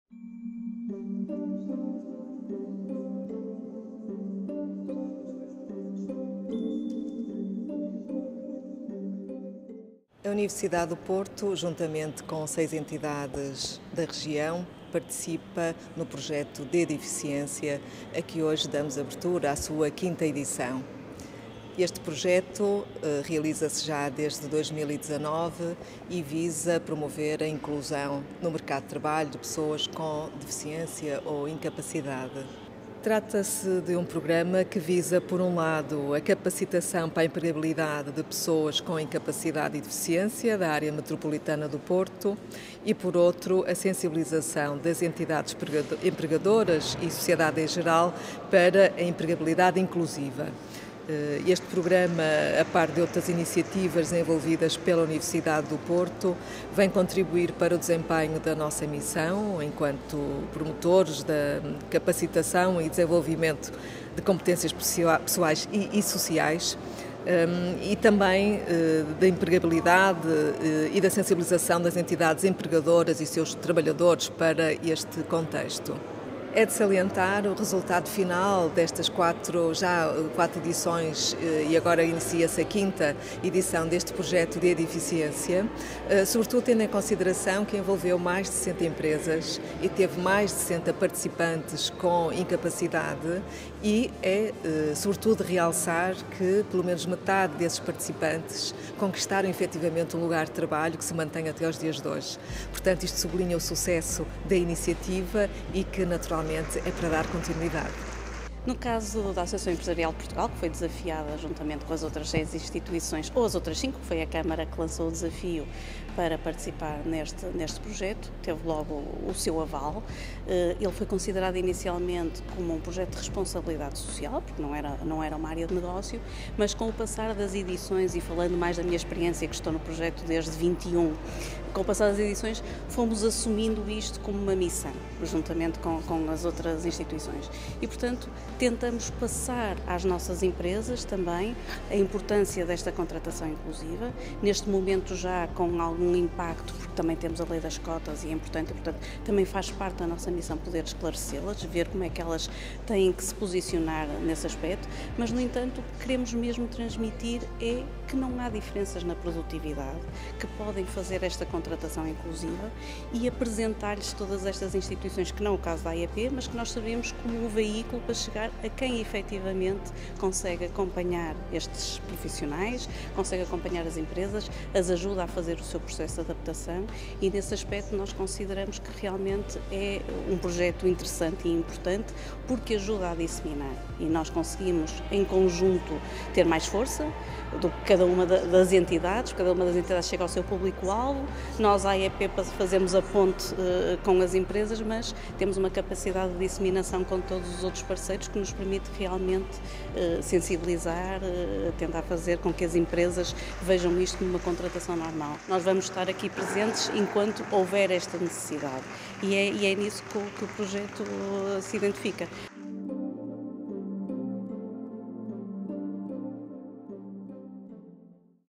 Projeto (D) de Eficiência apresentado na Universidade do Porto
O Projeto (D) de Eficiência é um Programa de inclusão de pessoas com deficiência e/ou incapacidade no mercado de trabalho – desenvolvido, desde 2019, num trabalho em rede com 7 entidades da cidade do Porto (a Universidade do Porto, a Associação Empresarial de Portugal, a Associação do Porto de Paralisia Cerebral, a Associação Salvador, a Câmara Municipal do Porto, o Instituto do Emprego e Formação Profissional e o Politécnico do Porto). Trata-se de um programa que visa, por um lado, a capacitação para a empregabilidade de pessoas com incapacidade e deficiência da Área Metropolitana do Porto e, por outro, a sensibilização das entidades empregadoras e da sociedade em geral para a empregabilidade inclusiva.

Ao longo de 4 edições anteriores já participaram no programa nas sessões de encontro entre profissionais e empresas mais de 65 empresas e no programa de capacitação mais de 60 pessoas com incapacidade e deficiência.
Este projeto possibilita a partilha de conhecimentos, experiências e práticas com outras instituições, permite-nos uma permanente atualização, aprendizagem, crescimento e evolução.

O evento  “Histórias de sucesso de inclusão e diversidade nas empresas” é relativo ao lançamento da 5ª Edição do Projeto D de Eficiência. Neste evento destacamos os testemunhos de pessoas com deficiência e incapacidade que foram contratadas no âmbito do projeto por empresas e testemunhos de empresas que contratam pessoas com deficiência e incapacidade

Este evento é também o 2ª Encontro da ComUNIDADE (D) de Eficiência, momento que junta Participantes, Empresas, Formadores e Amigos do Projeto, desde a 1ª Edição.